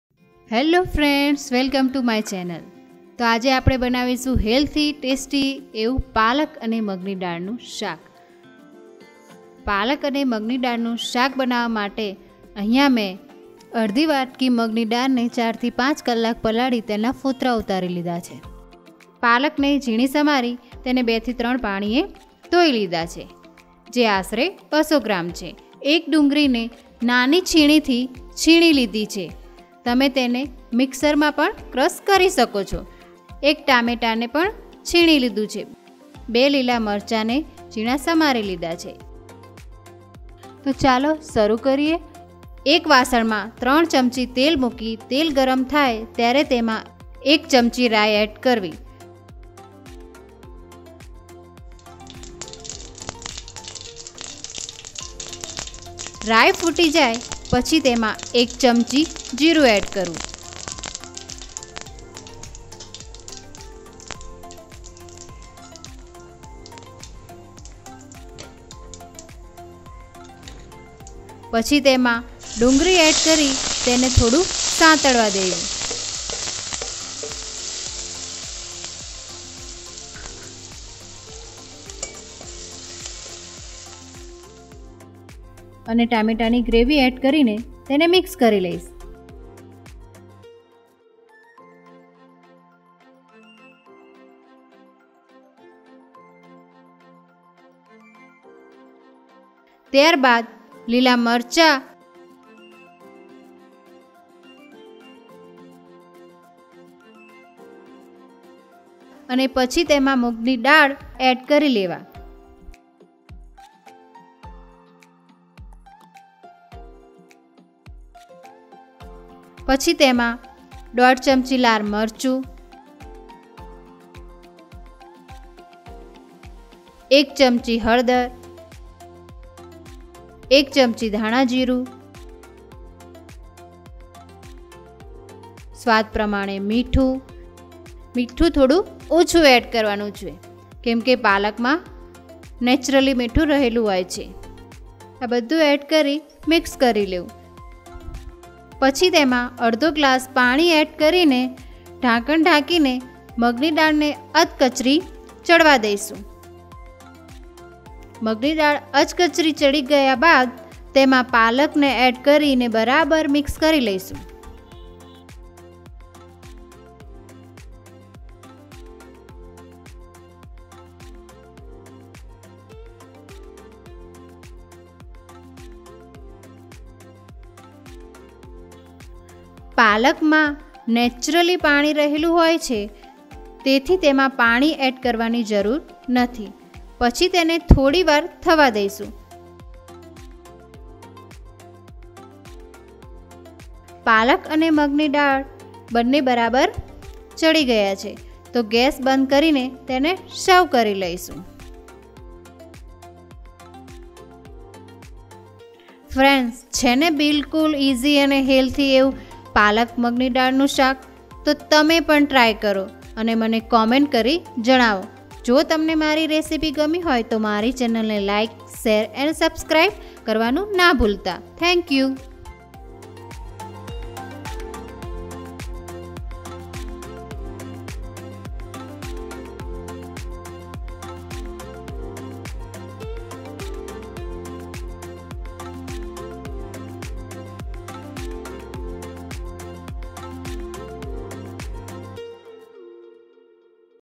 ટેસ્ટી અને હેલ્ધી પાલક અને મગની દાળનું શાક | Umi ki Rasoi | Gujarati Food | Healthy Food
ટેસ્ટી અને હેલ્ધી પાલક અને મગની દાળનું શાક Umi ki Rasoi | Gujarati Food

Ingredients:

1/2 Bowl Mung Dal
200 grams spinach
1 onion
1 tomato
2 green chillies
3 tsp Oil
1 tsp Mustard seed
1 tsp Cumin seed
1.5 tsp Red chilli
1 tsp Turmeric powder
1 tsp Coriander powder
Salt as per taste
1/2 Glass water